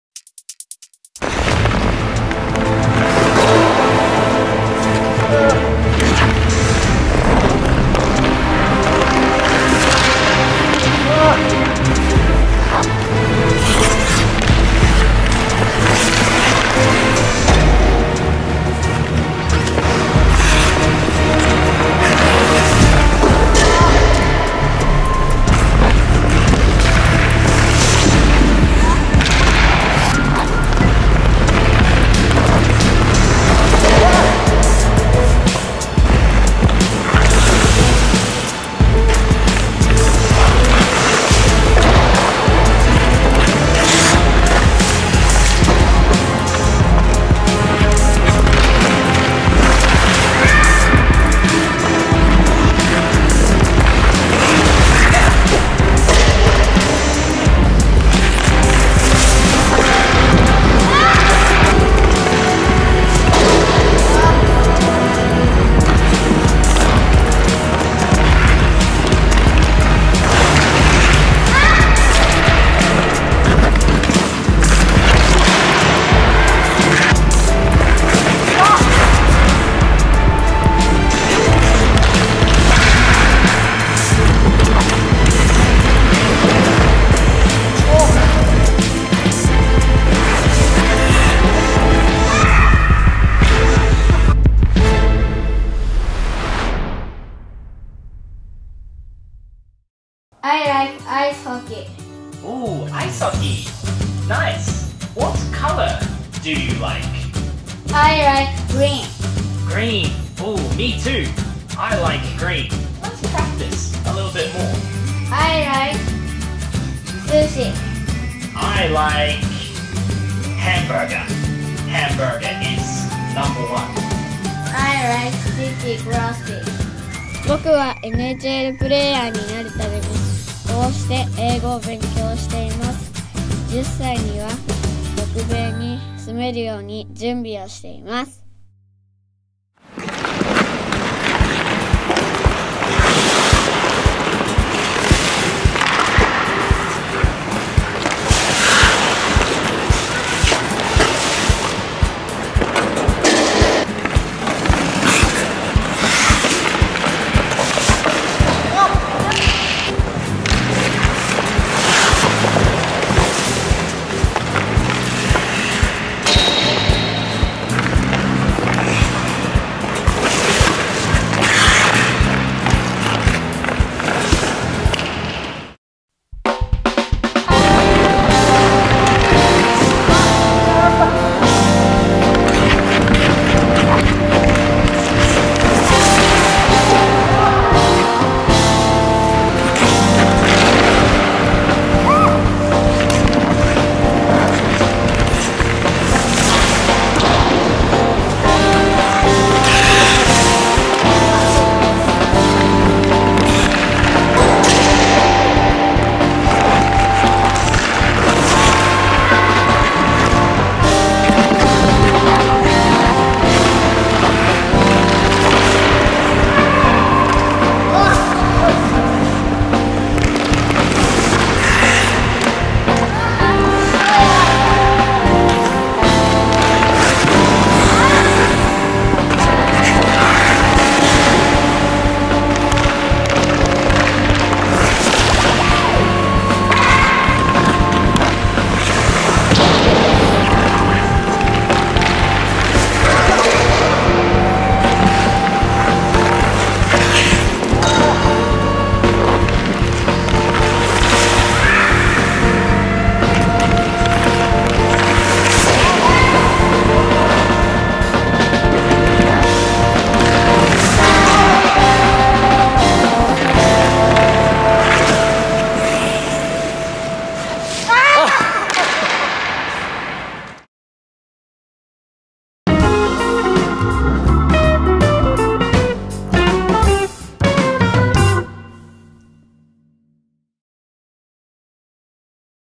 [Shootout Highlights] 8yr Hockey Kid vs Adult Goalie
Shootout with his team's goalkeeper coach. And it is a video of him studying English. He is working hard, dreaming of moving to North America in the future. 😸

8 Years Old / Novice Hockey Player

I am also a drummer. Please check it out.